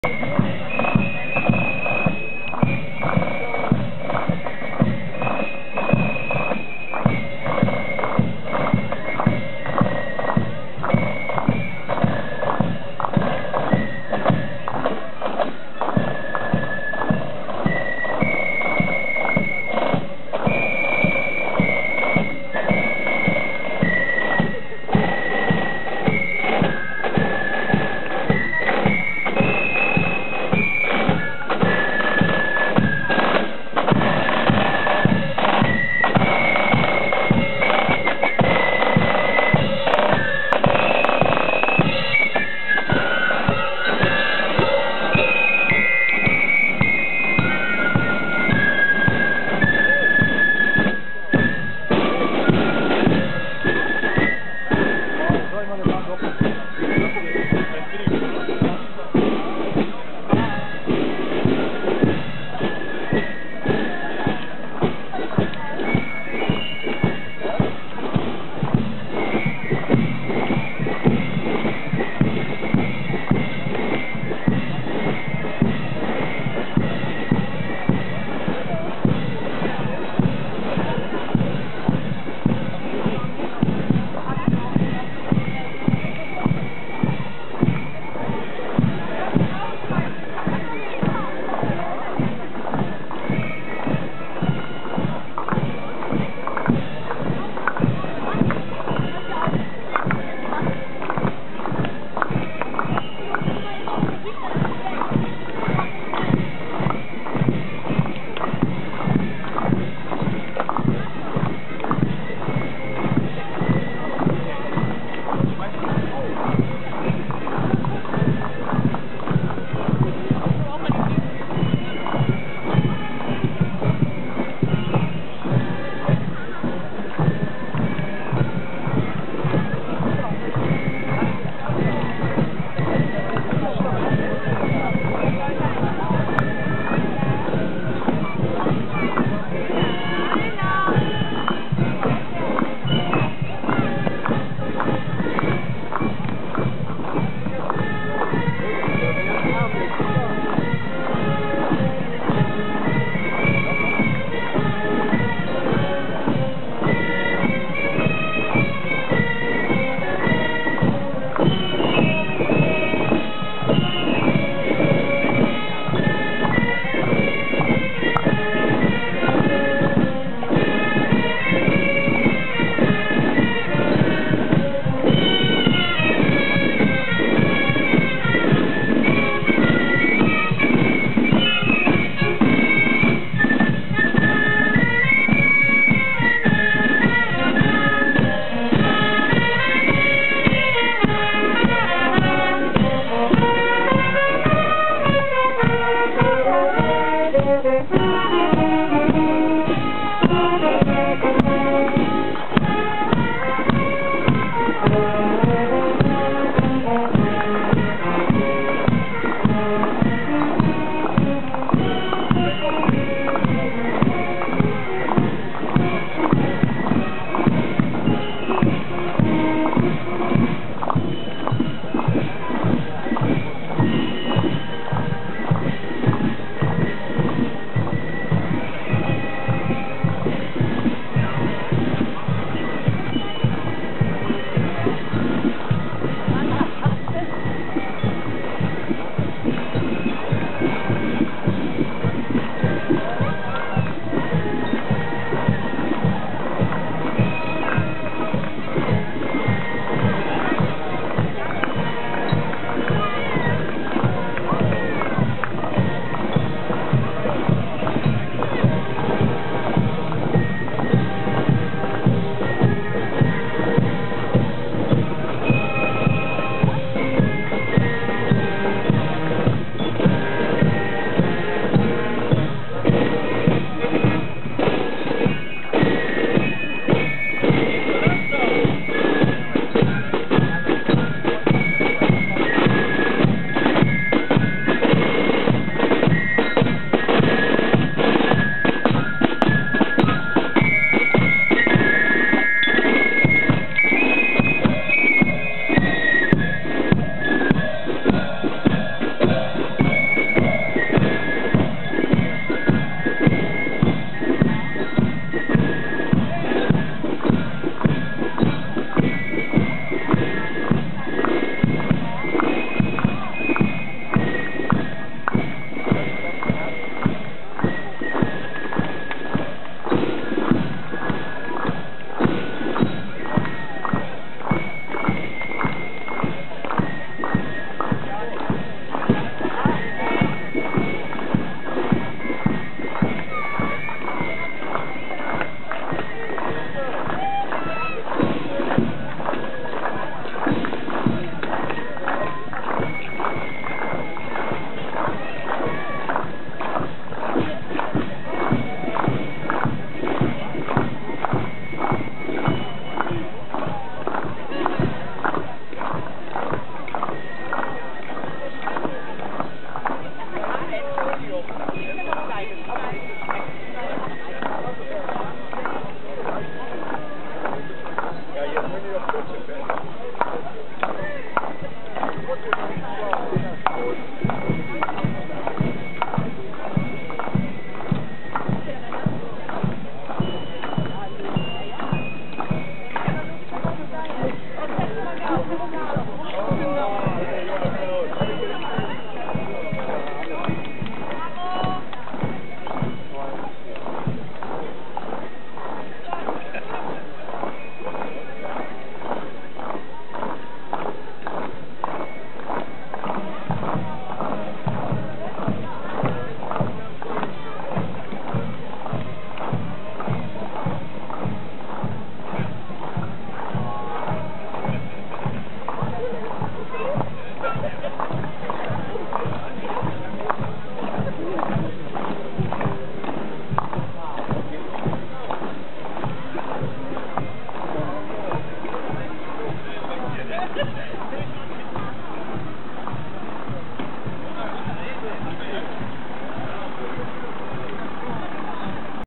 Klompenzug 2012 Bedburg Kaster Schützenfestmontag Brauchtum & Tradition 2011 - 2019
Jetzt ging der Klumpenzug los durch den Ort Bedburg Kaster
Schürzenjäger, 6. Jägerzug St. Martinus,  1. Marinenzug "Erftland Jonge", 2. Marinezug  "Blaue Jungs", 3. Marinezug "Große Freiheit",
2. Abteilung  Tambourcorps Kirch-Kleintroisdorf, Musikkapelle,  Alt Liblar,
1. Ulaner, Blaue Dragoner,  1. Garde Füsiliere, 2. Schill´ sche Offeziere, 3 `Schill` sche
Offeziere,  4. Schill `sche Offeziere.